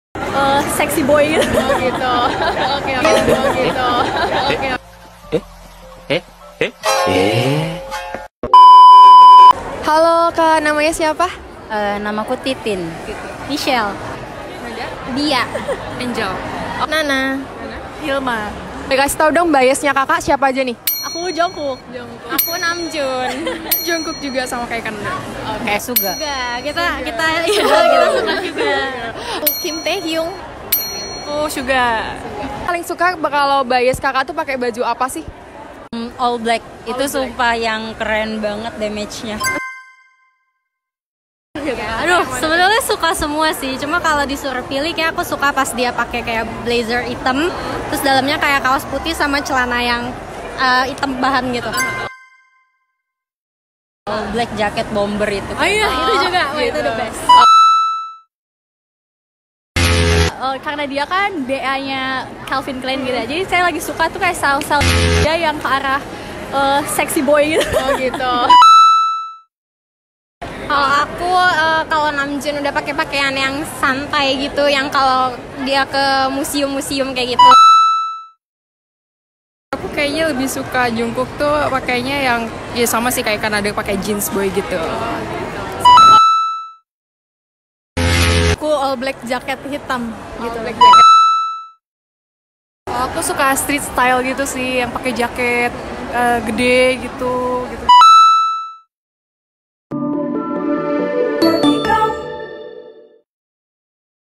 Outfit Favorite Bias Edisi BTS
Tonton video lengkapnya disini:

Apa outfit favorite bias kalian di BTS?! 😱🫣💖

Waktu main di BTS Pop Up Store kemarin, admin Wolipop sempet tanya-tanya nih sama para Army mengenai outfit favorite bias mereka! 🤩

Wolifriends yang juga Army bisa banget komen di bawah juga ya! 😍🔥